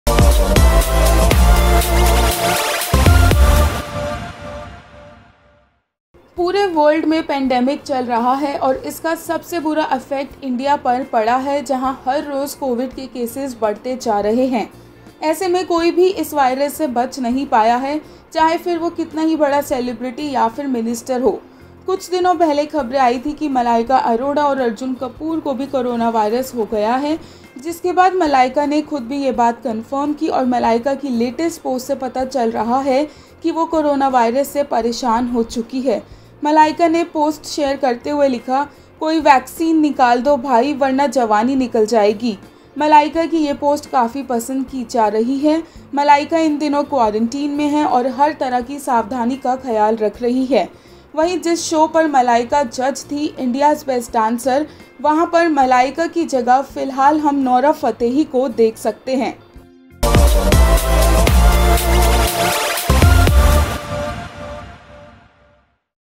Malaika Arora Shares Post On Coronavirus C0vid-19 Vaccine Says Please Someone Take Out A Vaccine
Malaika Arora Shares Post On Coronavirus Covid-19 Vaccine Says Please Someone Take Out A Vaccine